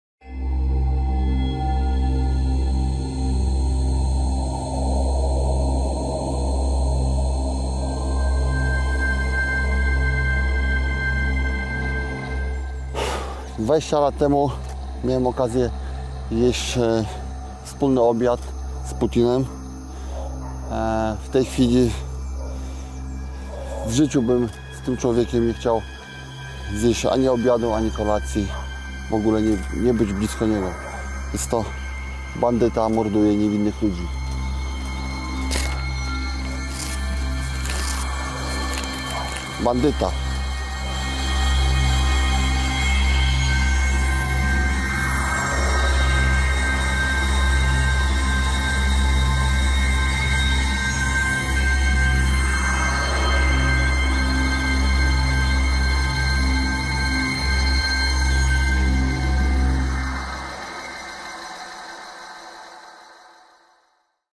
Mistrz olimpijski PALI ZDJĘCIE PUTINA! Paweł Nastula tłumaczy swój krok
Mistrz olimpijski w judo Paweł Nastula (52 l.) nie tylko popiera decyzję Międzynarodowej Federacji Judo IJF, która zawiesiła Władimirowi Putinowi godność honorowego prezydenta i ambasadora tej organizacji, lecz także wzywa do podjęcia zdecydowanych kroków celem położenia kresu najazdu Rosji na Ukrainę. Z tego powodu zdecydował się na radykalny krok.